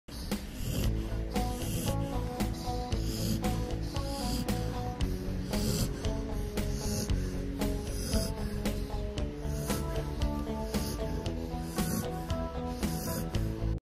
Golden Retrievers sleep sweetly on the road
Watch as the Golden Retriever puppies playfully explore the world alongside the charming Gordon Setter pup. Join in the joyous moments as the dogs have fun, showcasing the playful nature of Golden Retrievers and the adorable antics of the Scottish Gordon Setter puppy. Experience the excitement of travels with our canine companions. Don't miss out on the heartwarming moments and the entertaining escapades of these Golden Retrievers and Scottish Gordon Setter.